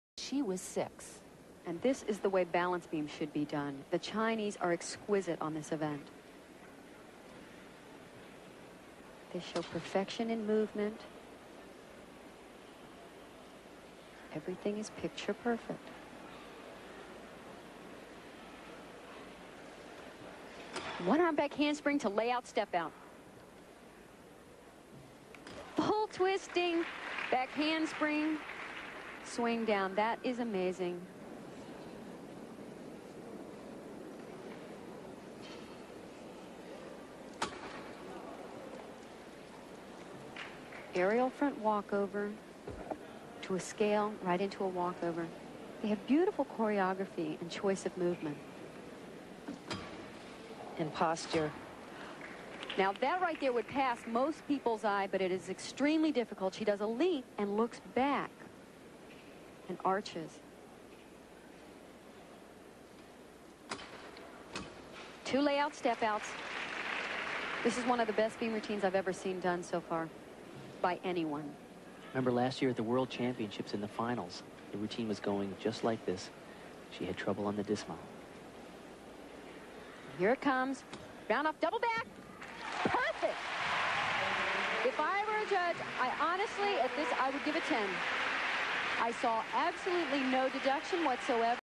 Yang Bo (CHN) 1990 World Cup BB - All Around
This is quite possibly the greatest and most beautiful beam routine we've seen. Yang Bo is simply exquisite to watch - sharp, yet fluid, strong, yet graceful, difficult, yet appears effortless. Where the deduction came from I'll never know!

Final Score: 9.987. Where is the deduction??